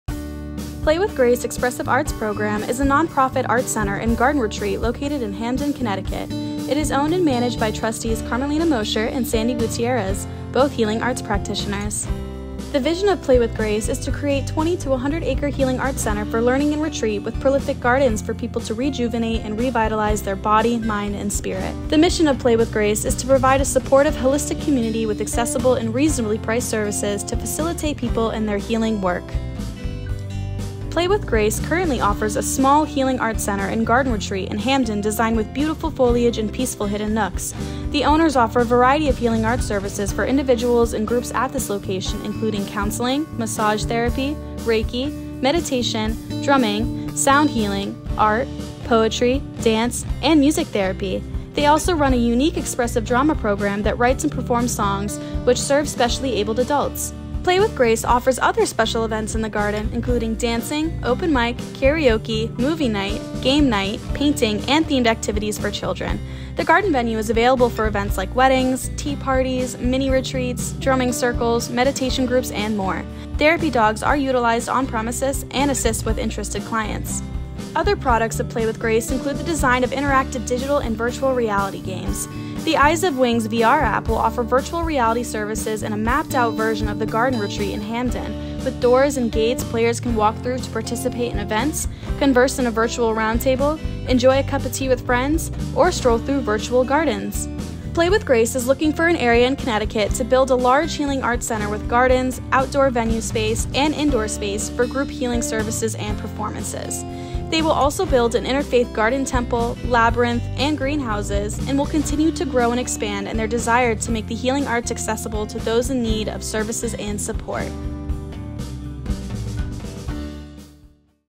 Play with Grace
Play with Grace expressive arts program is a nonprofit Arts Center & Garden Retreat located in Hamden, Connecticut. It is owned and managed by trustees Carmelina Mosher and Sandi Guttierrez, both healing arts practitioners.